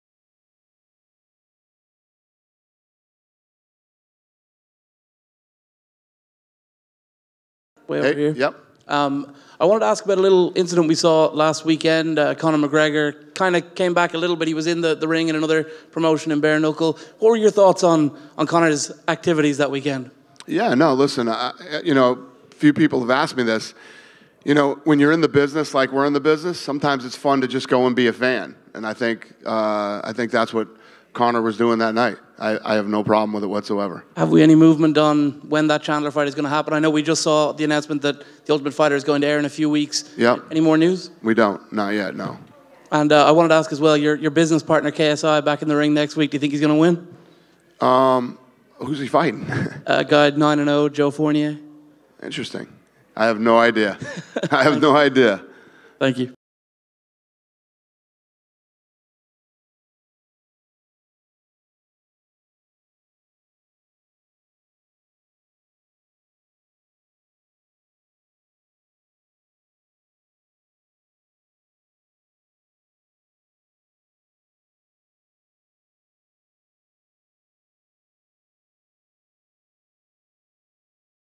Dana White Reacts to Conor McGregor getting in BKFC ring
Dana White gives his thoughts on Conor McGregor getting in the Bare Knuckle boxing ring and facing off with Mike Perry